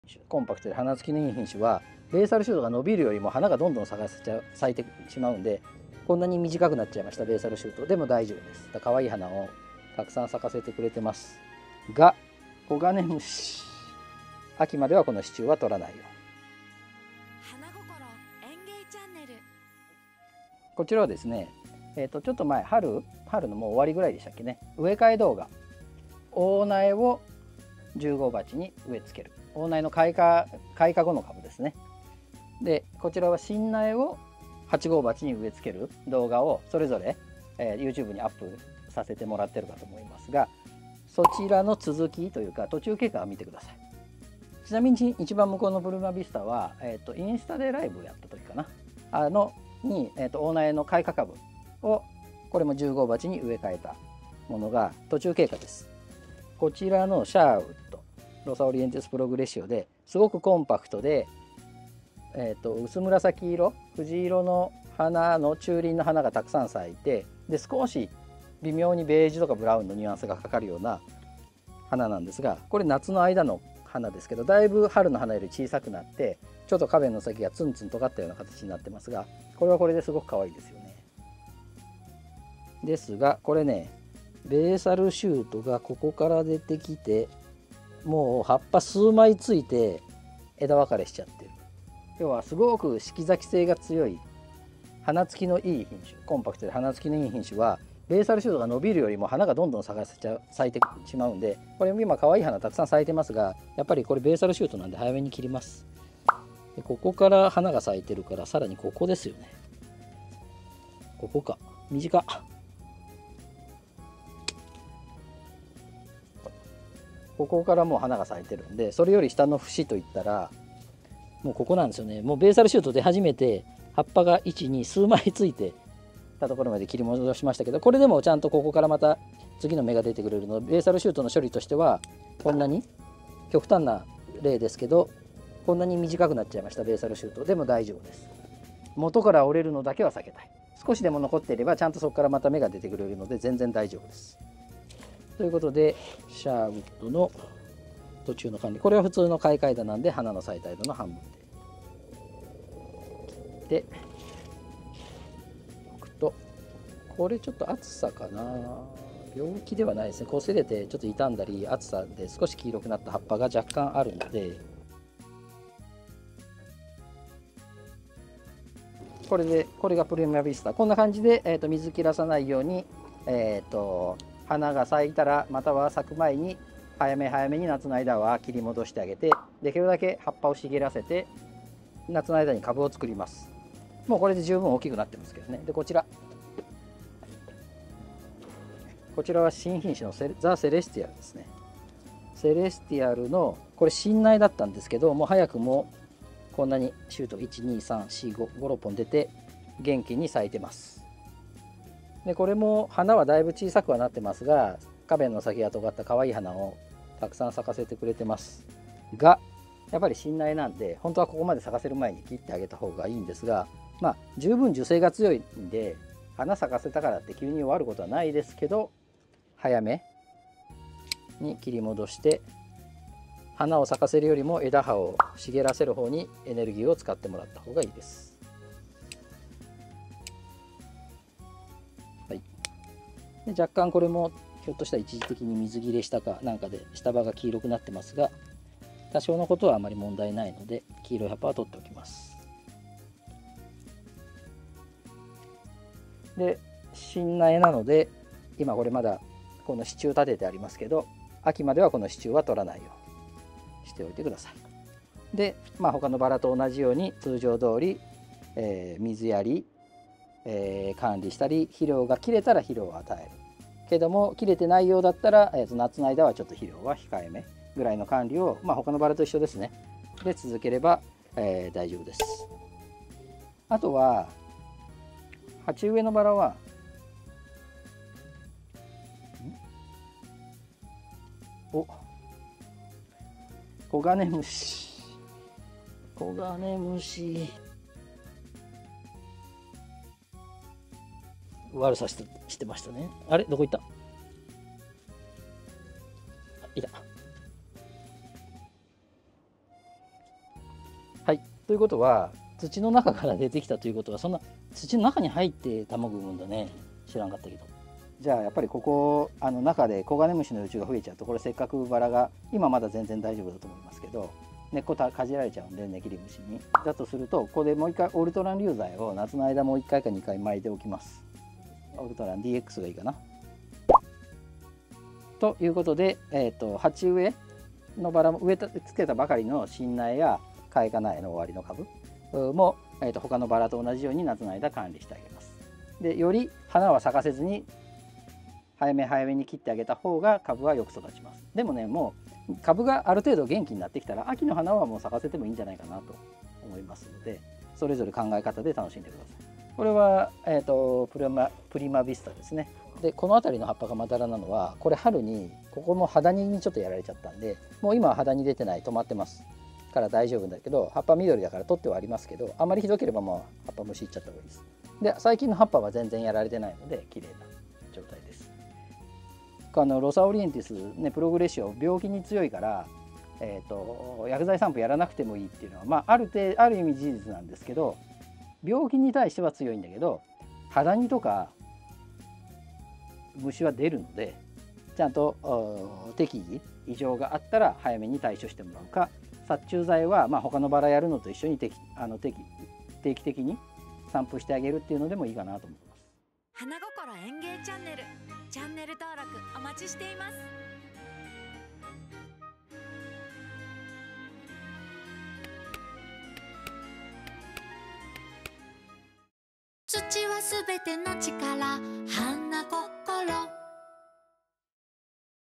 春からの成長ぶりを見てみましょう。8月の大苗・新苗の様子
【はなはなショップ】農場から新鮮な苗を直送します